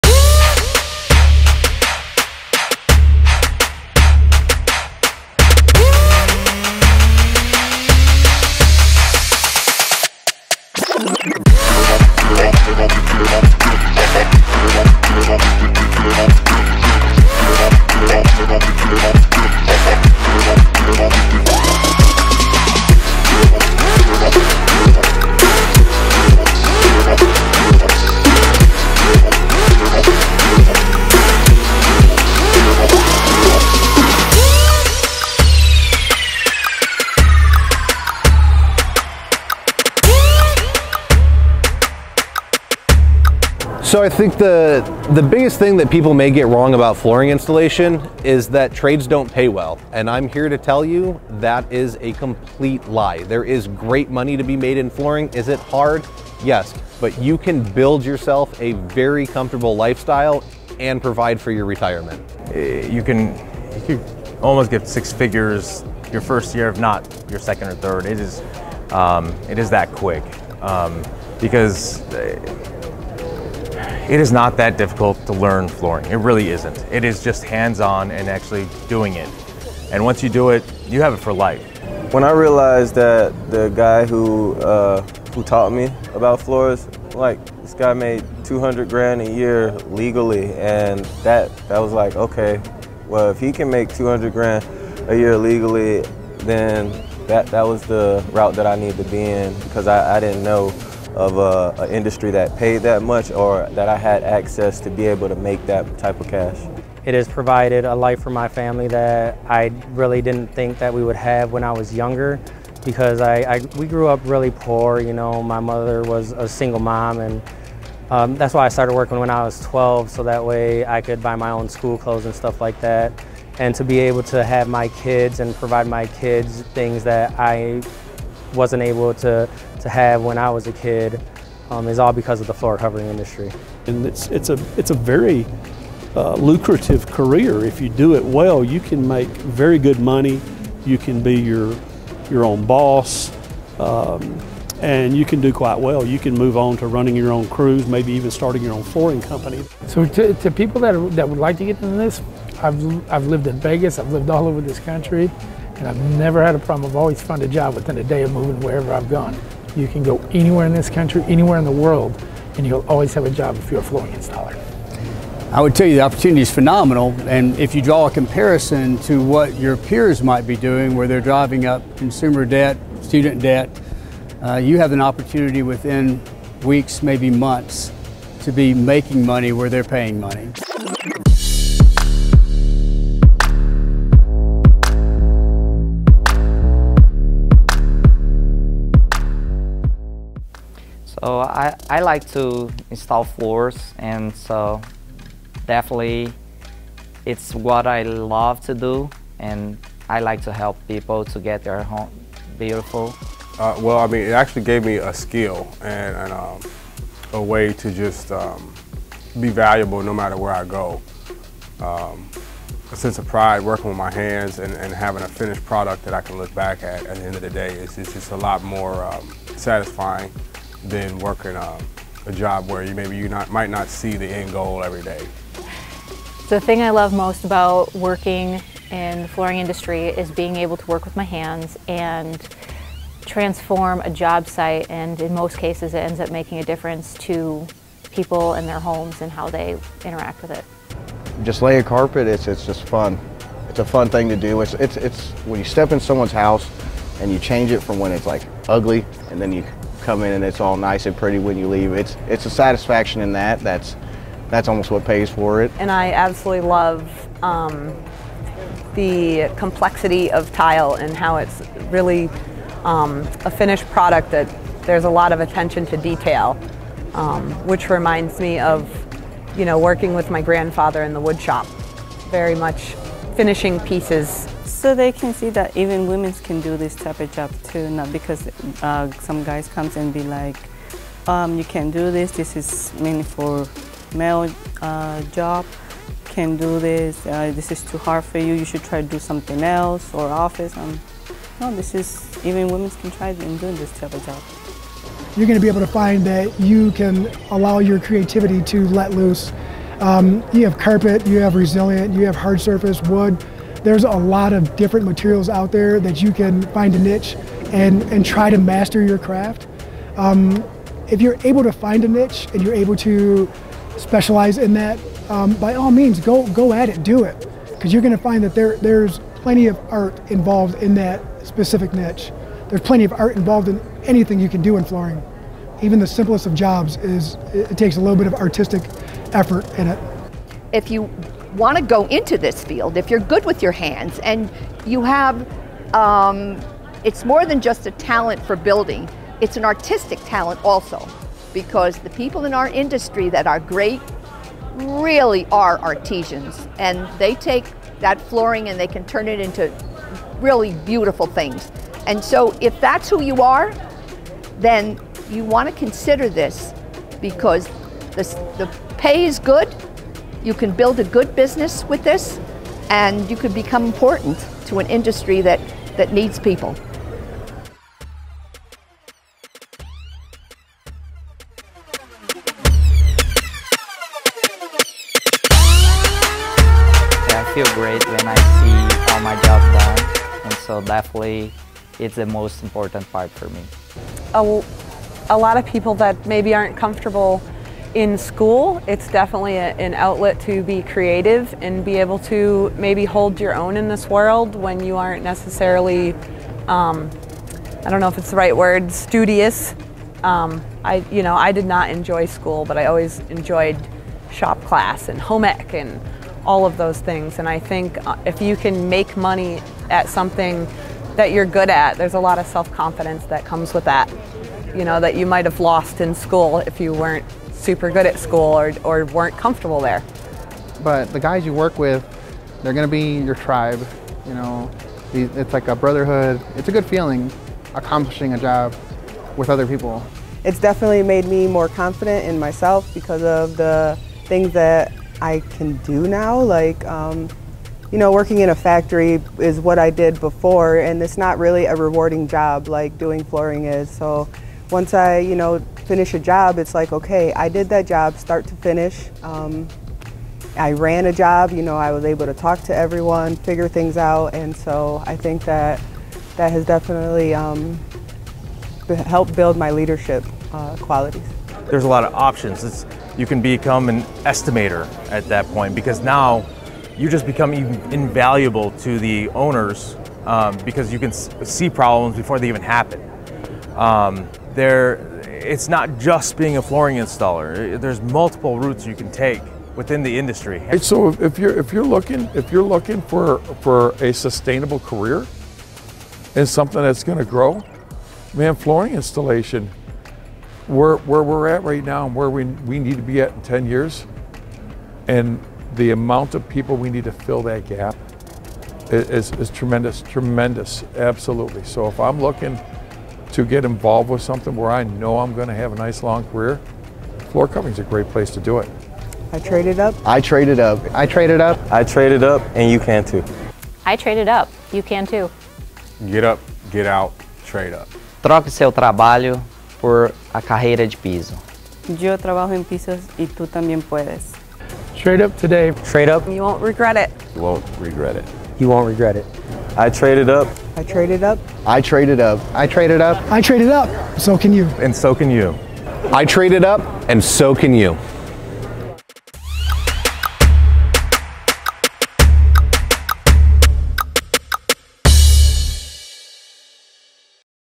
Trade Up to a Career as a Flooring Craftsman
The flooring industry is looking for creative people who love to work their hands. As a flooring craftsman, you'll find opportunities for career growth, financial freedom, and independence.

Here from craftsmen and craftswomen who have already traded up also well as flooring industry representatives and learn more about how the FCEF is helping more people trade up with scholarships for training.